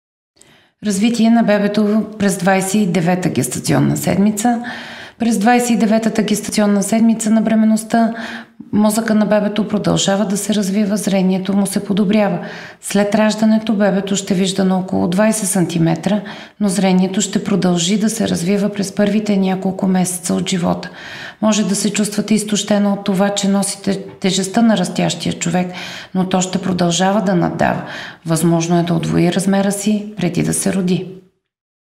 Развитие на бебето през 29-тата гестационна седмица
Онлайн училище за родители - Dechko.bg, ви представя д-р Светла Иванова. Тя ще ни запознае с промените, които настъпват при нашето бебе и какво е влиянието му върху майчиния организъм през 29-тата гестационна седмица.